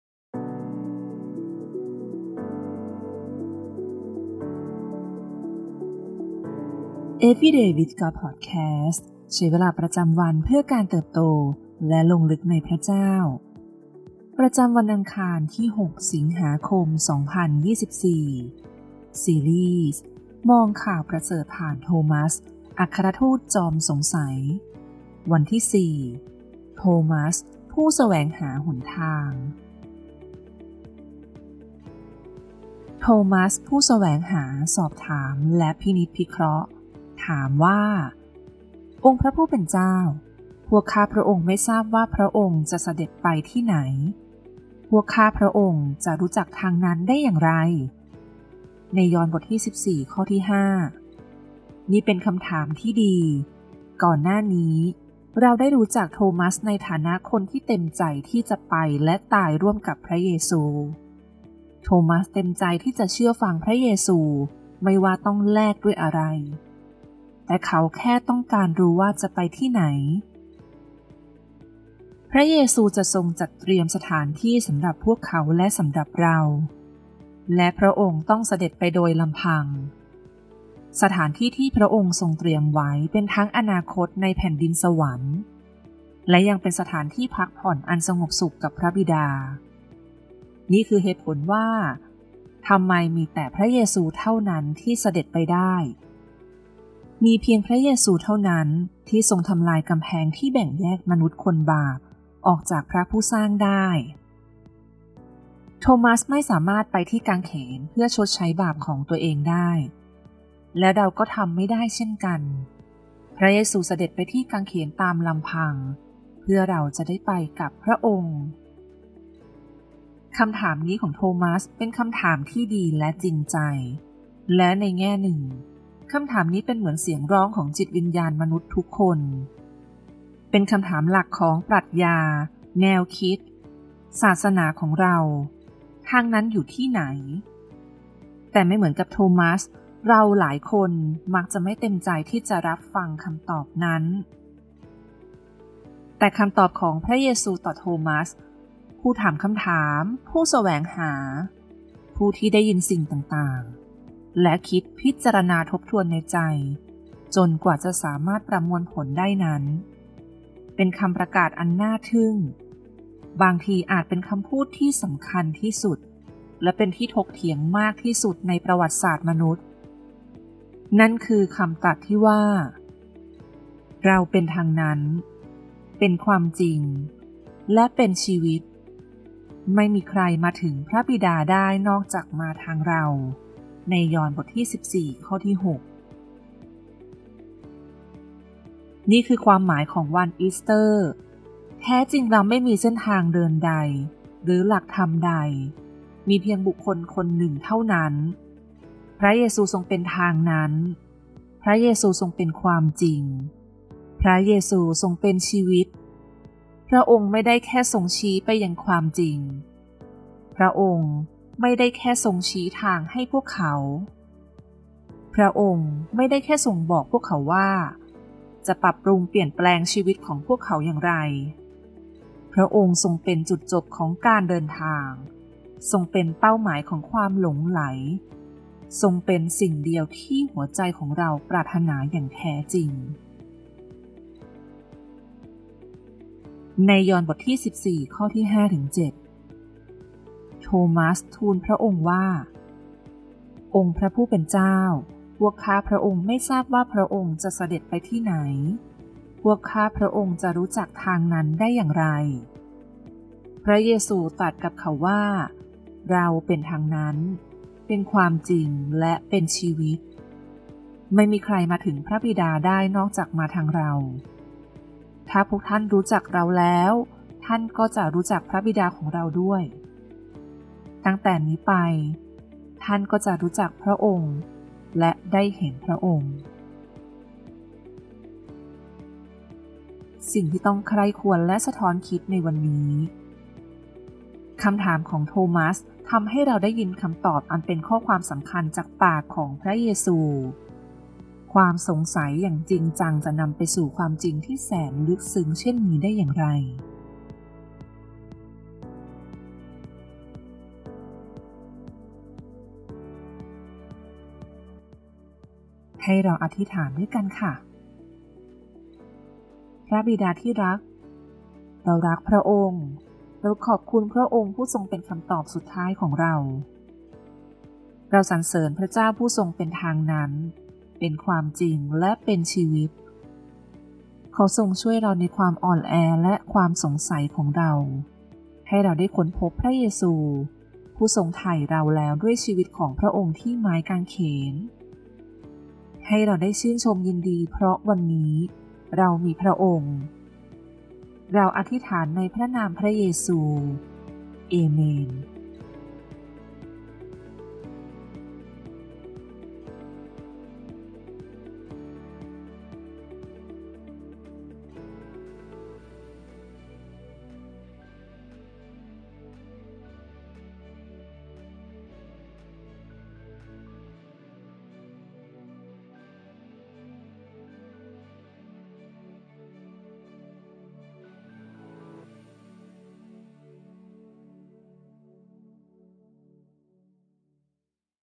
6/8/2024 Every Day with God | ซีรีส์ : มองข่าวประเสริฐผ่านโธมัส อัครทูตจอมสงสัย | วันที่ 4/7
ประจำวันอังคารที่ 6 สิงหาคม 2024

ซีรีส์ : มองข่าวประเสริฐผ่านโธมัส อัครทูตจอมสงสัย
วันที่ 4 โธมัสผู้แสวงหาหนทาง

OVERVIEW :
ในแผนการอ่านตลอดเจ็ดวันนี้ แดเนียล ดาร์ลิง ผู้เขียน ได้ตามรอยเรื่องราวของอัครสาวกโธมัส  เราจะได้รู้จักเรื่องราวชีวิต อุปนิสัย และเหตุผลว่าทำไม เขาจึงมักถูกตราหน้าอย่างไม่เป็นธรรมว่าเป็น “โธมัสจอมขี้สงสัย”  แผนการอ่านนี้จะช่วยหนุนใจทุกคนที่กำลังปล้ำสู้กับความเชื่อของตนว่า “ข้าแต่พระเจ้า ข้าพเจ้าเชื่อ และขอโปรดช่วยในส่วนที่ขาดอยู่ด้วยเถิด”

บทใช้เวลากับพระเจ้านี้แปลและเรียบเรียงจากแผนการอ่าน The Gospel for Doubters, Good News Through the Eyes of Thomas โดย Daniel Darling และสำนักพิมพ์ Moody Publishers